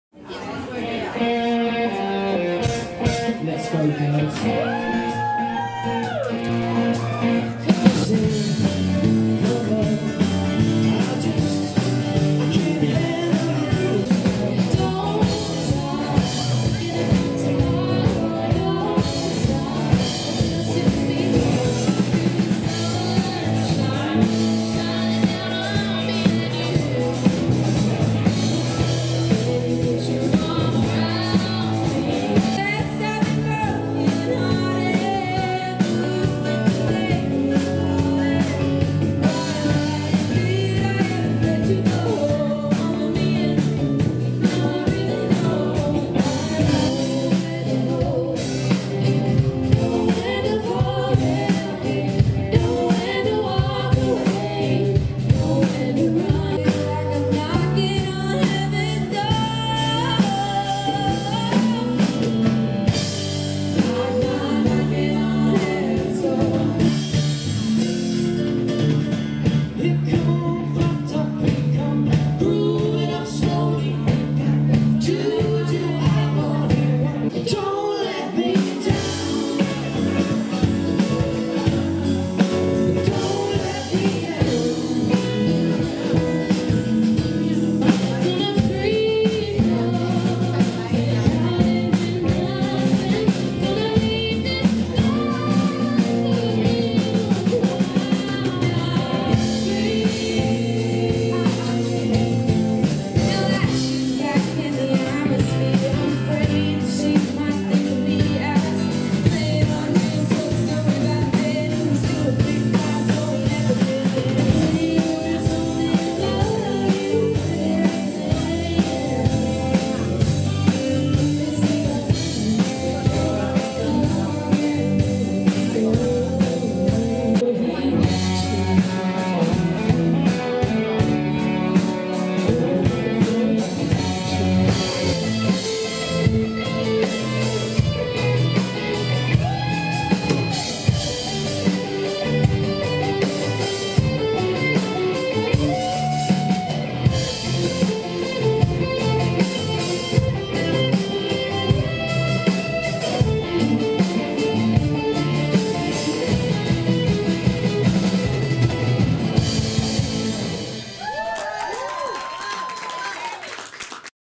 Crooked Line Band Promo
Crooked Line are a 5 piece cover band from Brisbane, playing classic and country rock music.